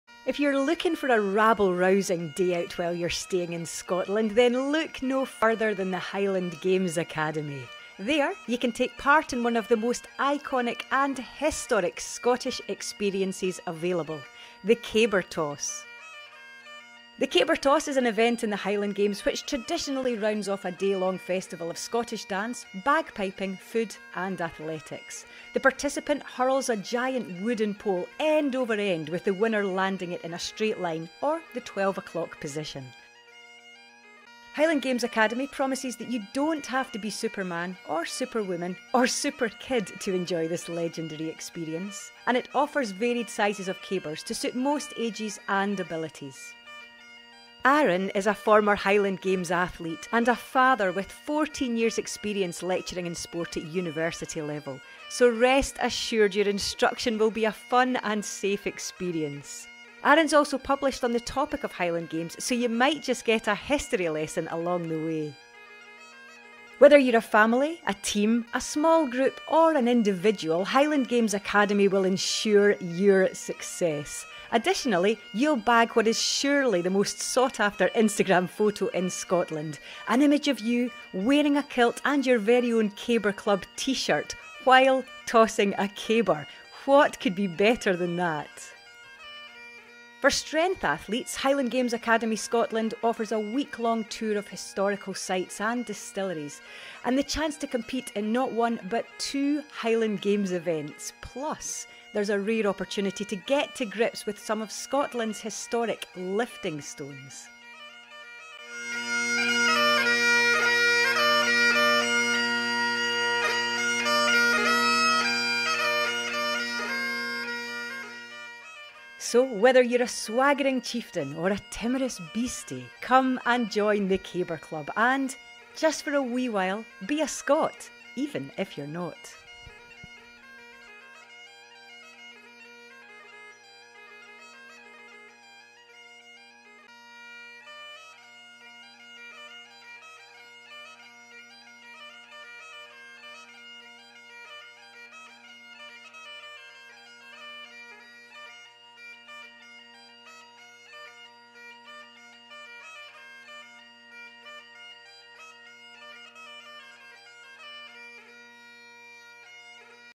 Highland Games Academy Scotland Promotional Video
Scottish Highland Games Caber Tossing for domestic and international tourists, Scouts earning badges, parties, stag parties, hen do's, team building, corporate team building, etc.